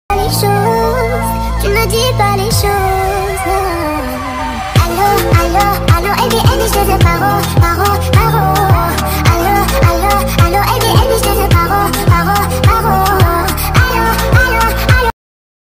Liatris ligulistylis, also known as meadow blazingstar, is irresistible to Monarch butterflies.
This content doesn’t belong to TimeToLaunch, it is edited and shared only for the purpose of awareness.
Regards,
TimeToLaunch